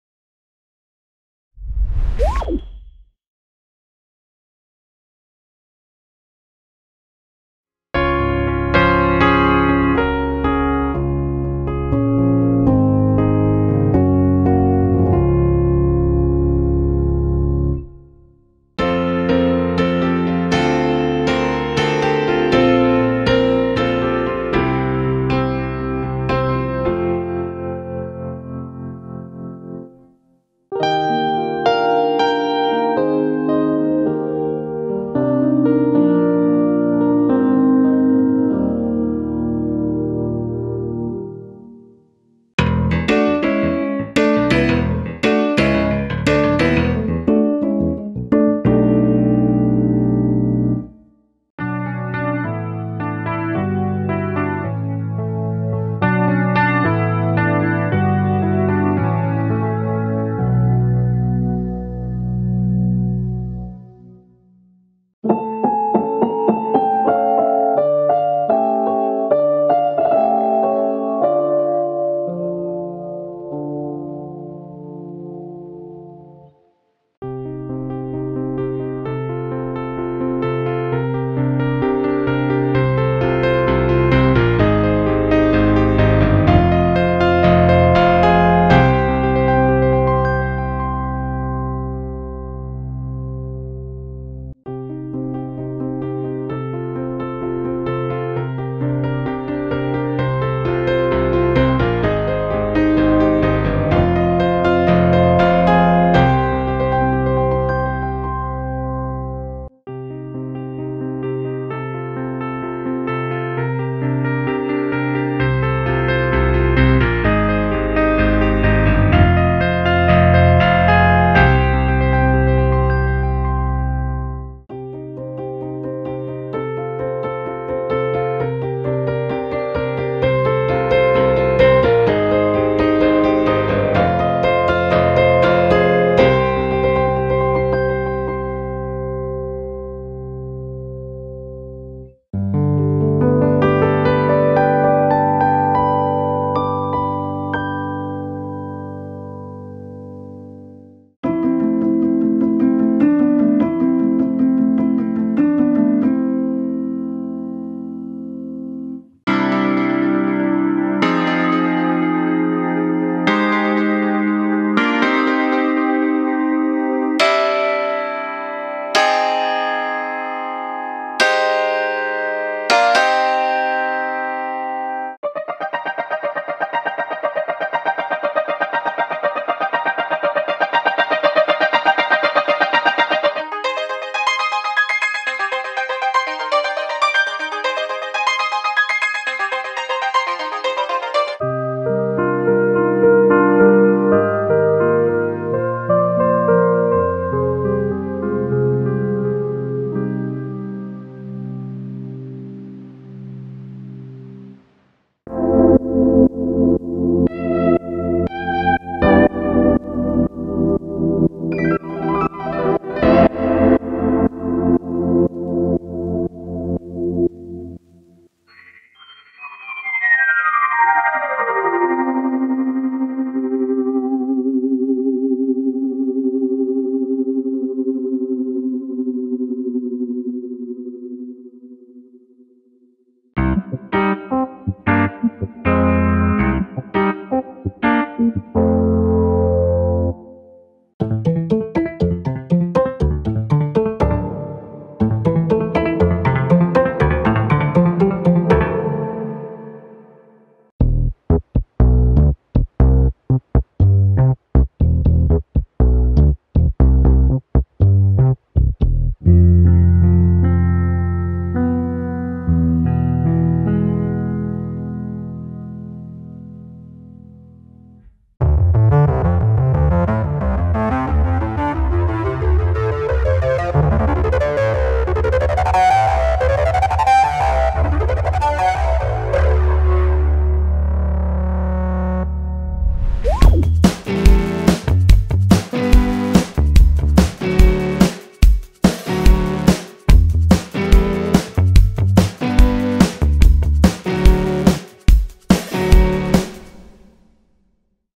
Addictive Keys - Electric Grand Preset Walkthrough
Listen through the presets of Addictive Keys: Electric Grand in this walkthrough video.